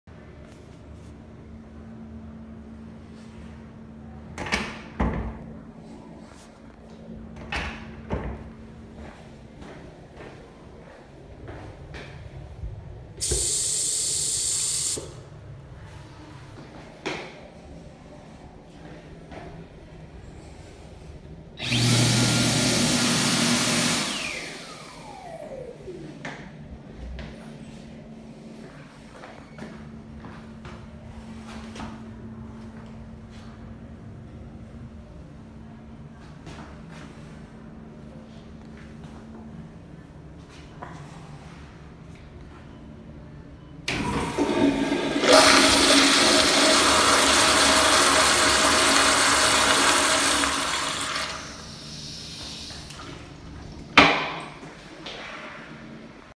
3. Kroger Kentucky Family Restroom (Full Shoot)
(Recorded Date: July 2, 2020)

UPDATE As of April 23, 2021: This Madera now has a Zurn AutoFlush Sensor.

This restroom was filmed at the same
as the last video.

The fixtures are: An American Standard Madera Toilet, A Counter Top Sink, Kimberly-Clark Paper Towel Dispenser and a Dyson Airblade-V Hand Dryer and that is everything I seen in this restroom.